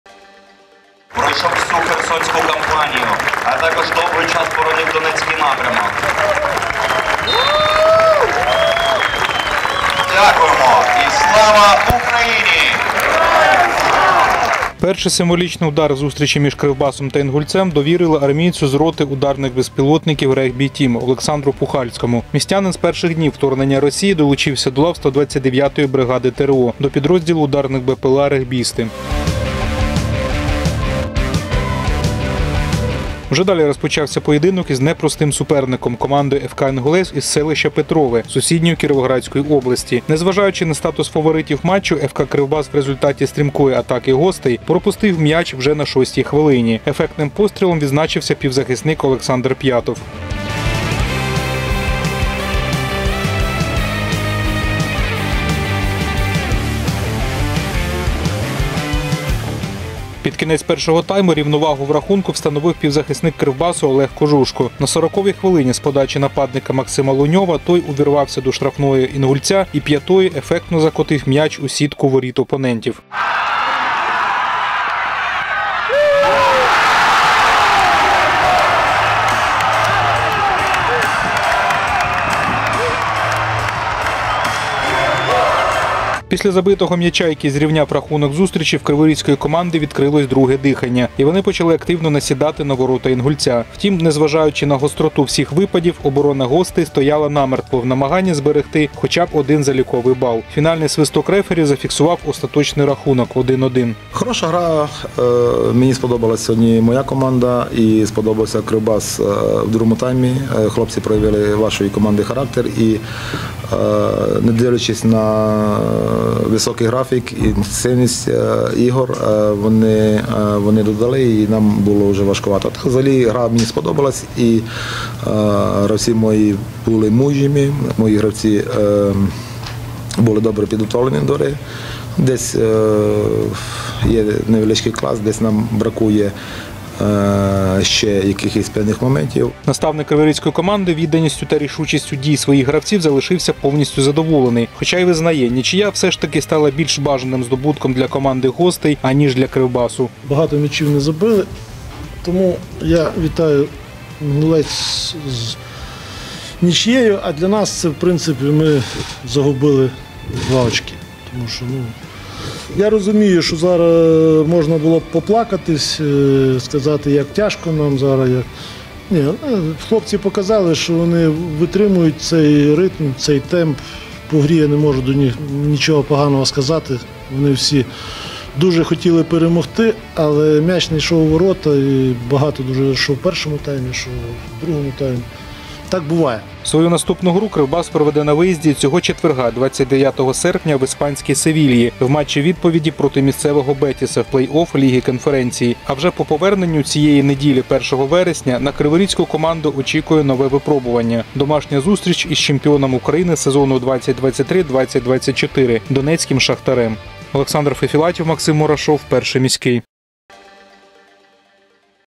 ФК «Кривбас» - ФК «Інгулець» - 1:1
Бойова нічия. В домашньому матчі ФК «Кривбас» поділив залікові бали із гостями з команди ФК «Інгулець» (Петрове, Кіровоградська область) – 1:1. Зустріч двох географічних сусідів прогнозовано видалася насиченою боротьбою та чисельними атаками на ворота опонентів. Глядачі побачили два забитих м'ячі, і ще більше – нереалізованих моментів в червоно-білих. За подіями на газоні стадіону «Гірник» спостерігали наші кореспонденти.